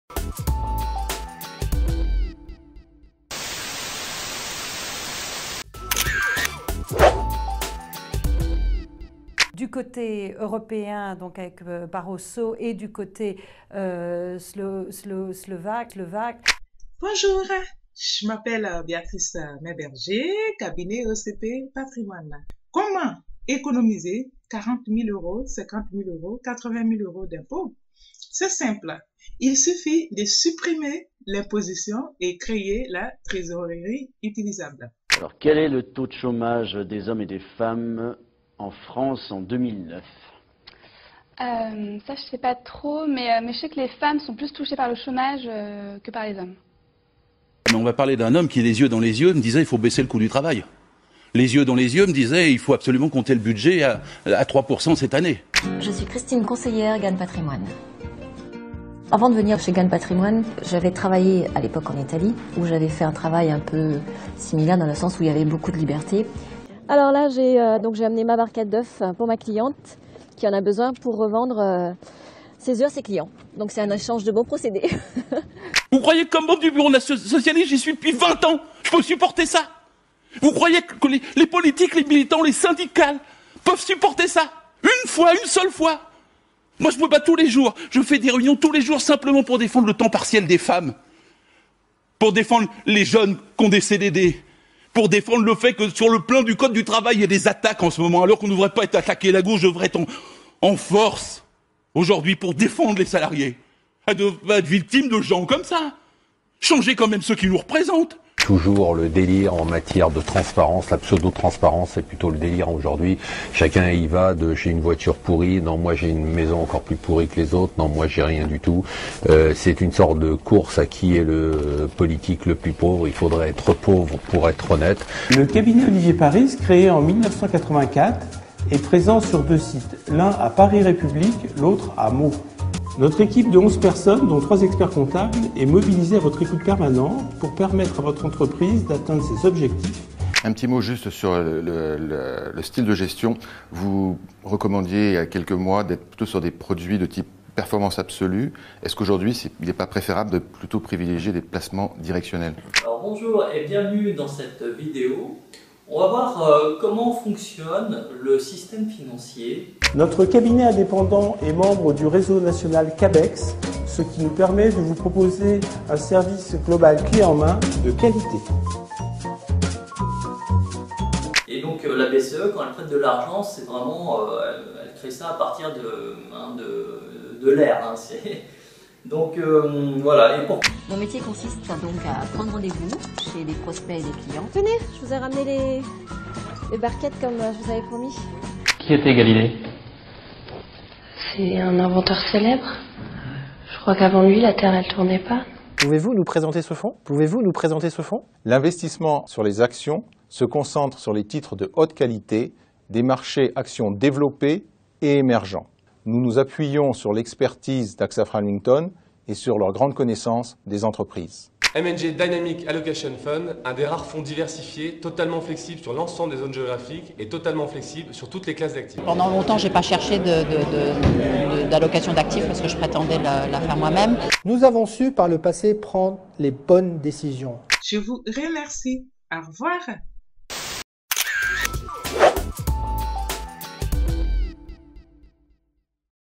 Le Zapping de la Finance
Le meilleur et parfois le pire des vidéos publiées sur Internet par les asset managers et les CGPI. C'est drôle, c'est instructif. Parfois pathétique. A déguster sans modération.